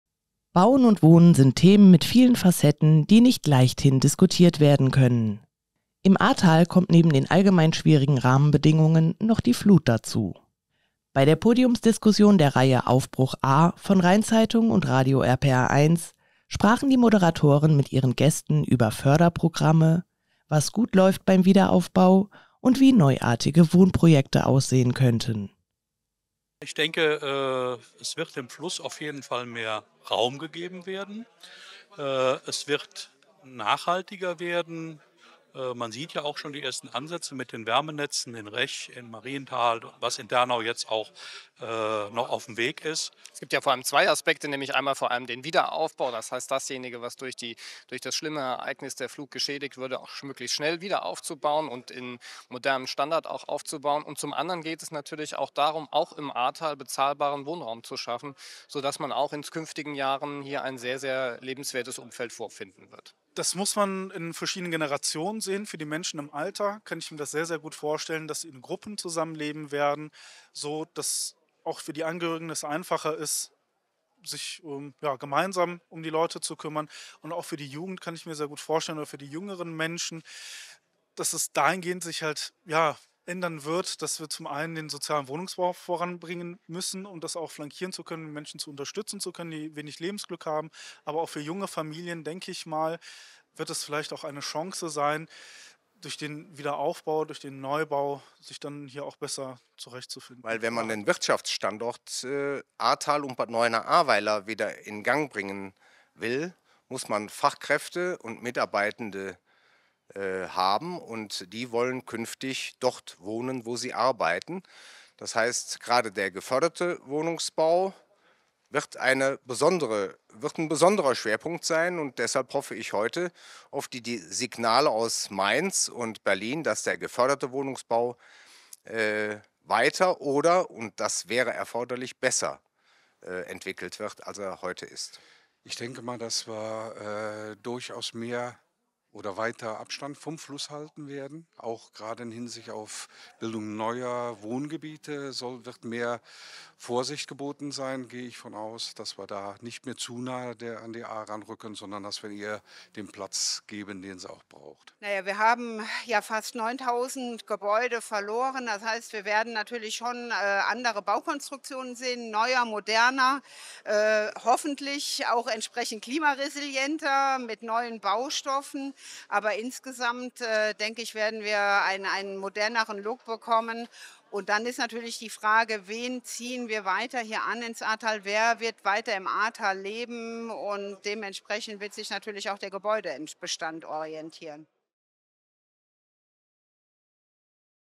RZ-Podiumsdiskussion: Was das Wohnen an der Ahr schwierig macht
Wohnen und Bauen – das sind Themen mit vielen Facetten, die nicht leichthin diskutiert werden können. Im Ahrtal kommt neben den allgemeinen schwierigen Rahmenbedingungen wie gestiegenen Baukosten, der Zinssituation, der Suche nach Investoren und der angespannten Situation auf dem Mietwohnungsmarkt die Flut dazu. Rund zehn Prozent der flutbetroffenen Wohnungen in Bad Neuenahr-Ahrweiler sind noch nicht wieder hergestellt. Das verschärft die Situation zusätzlich. „Bezahlbarer Wohnraum ist die soziale Frage der Republik und nicht, ob man straffrei haschen kann“, brachte es Kreisstadt-Bürgermeister Guido Orthen auf den Punkt. Und doch bleiben viele Menschen optimistisch und positiv eingestellt. Das überraschte bei der Podiumsdiskussion der Reihe „Aufbruch Ahr“ von Rhein-Zeitung und Radio RPR1. am Donnerstagabend auf der Kleinen Bühne im Kurpark zum Thema „Wie verändert sich das Wohnen im Ahrtal?“. Gesprochen haben die Moderatoren Lars Hennemann, Chefredakteur der Rhein-Zeitung, und Jens Baumgart, Info-Chef von RPR.1, mit ihren Gästen über Förderprogramme. Diskutiert wurde aber auch, was das Ahrtal noch braucht und woran es hakt, dass der Wiederaufbau und die Ausweisung neuer Baugebiete schneller vonstattengehen.

🎥Sascha Ditscher
📄Celina de Cuveland
🎬Svenja Wolf & Jens Weber
🎤Svenja Wolf